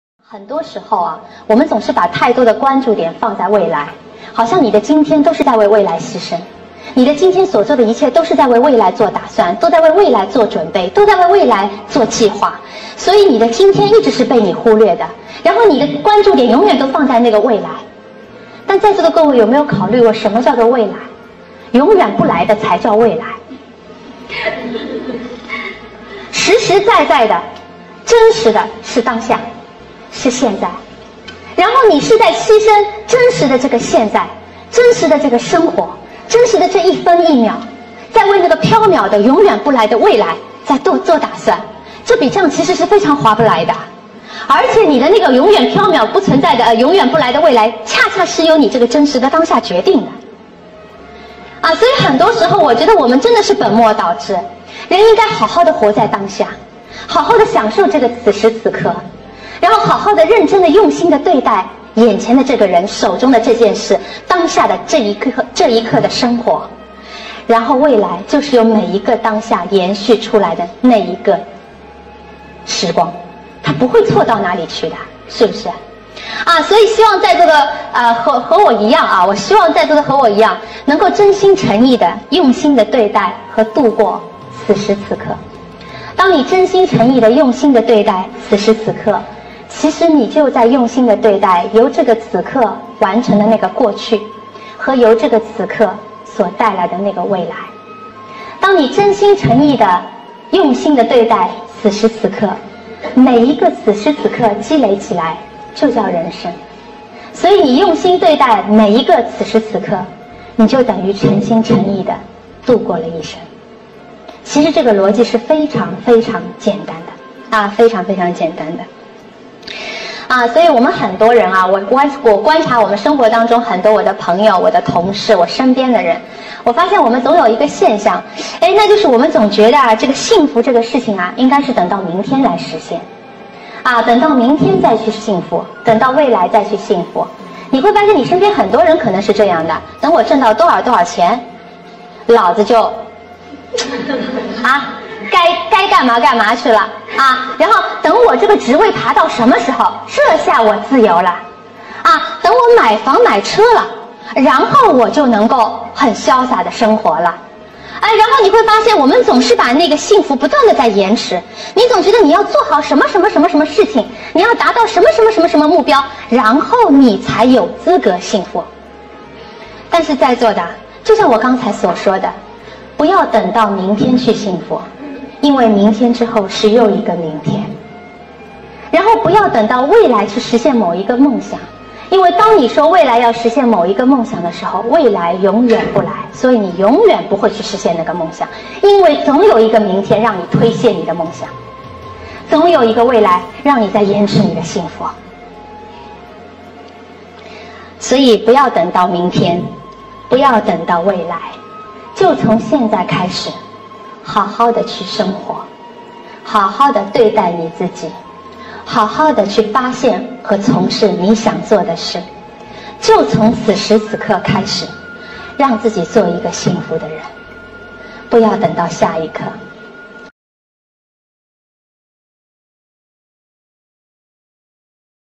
幸福哲学：什么叫未来？永远不来的才叫未来，为你解读过去，现在，未来~
幸福哲学：什么叫未来？永远不来的才叫未来，为你解读过去，现在，未来的关系，最重要的幸福就是活在当下